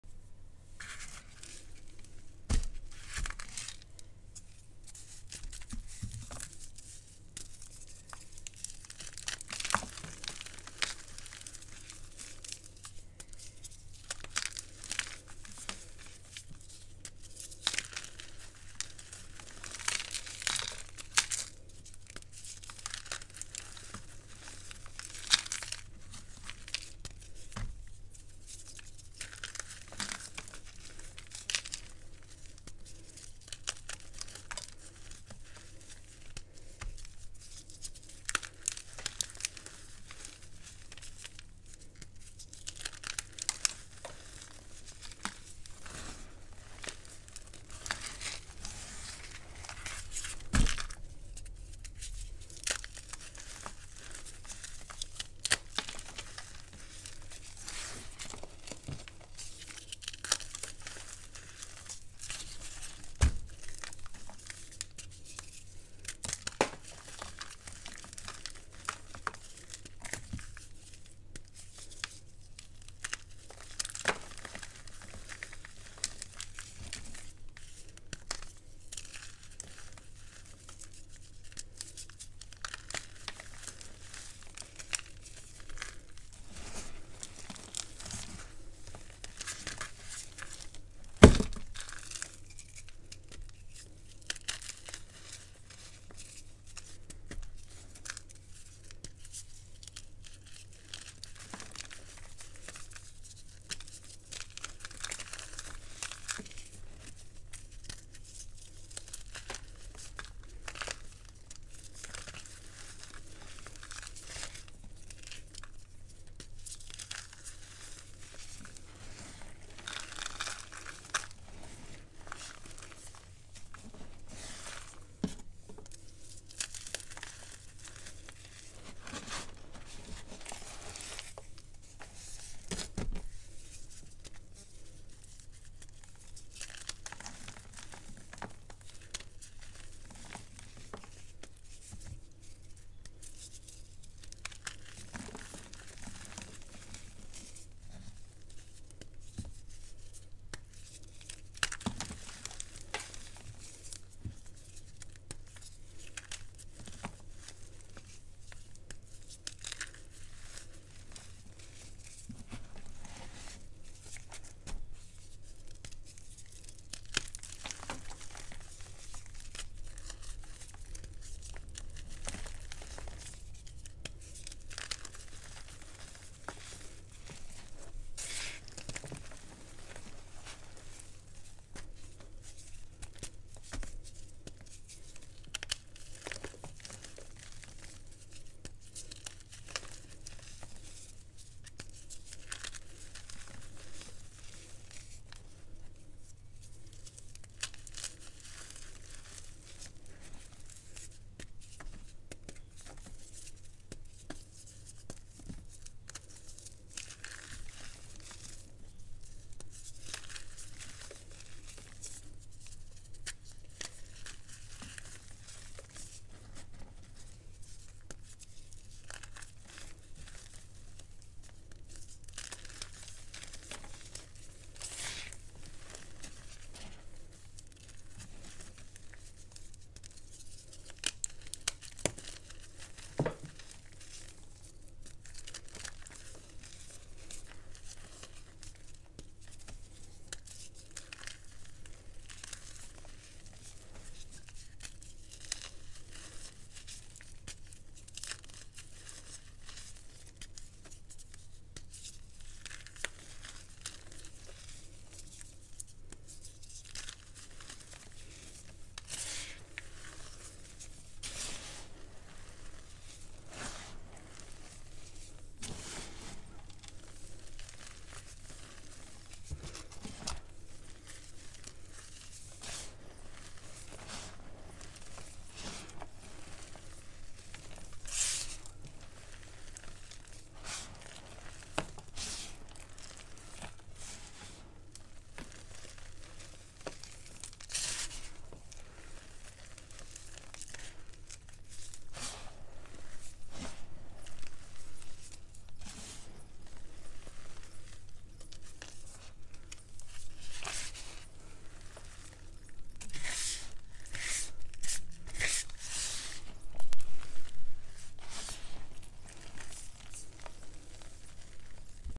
Satisfying Giant Green Gym Chalk Slab🍀🌴💚
Hi guys! I’m sorry to those of you who read the description from yesterday vid before I edited it. It made zero sense. I was literally typing in my sleep 😂 I try to upload real early & I won’t be awake yet.  My next video will be 6 blocks Ader gym chalk & cornstarch paste. Be good to yourself & others. Peace ✌🏼Love 💕& Asmr.